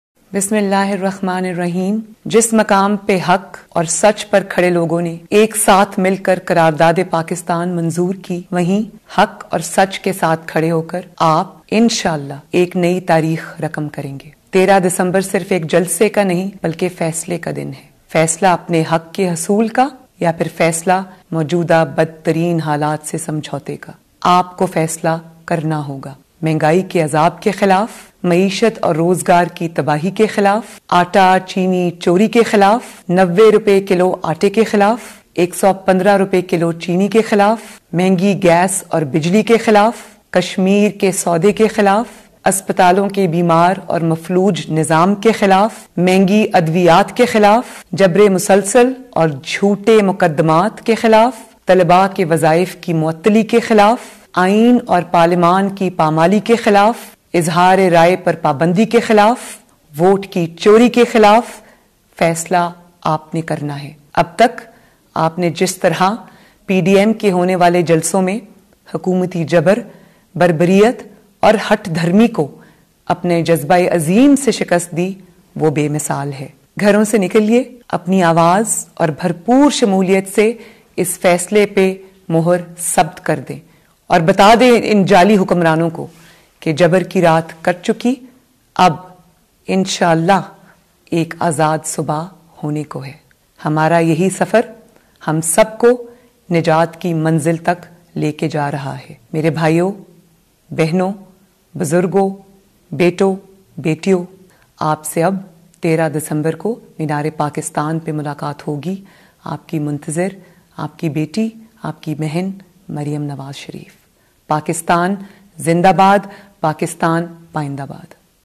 PDM Lahore Jalsa | PMLN Maryam Nawaz Important massage to Supporters | 11 December 2020
Maryam Nawaz Sharif's message to the people of Lahore. She urges people of Punjab, especially Lahore, to attend PDM's Lahore Jalsa at Minar-e-Pakistan on December 13.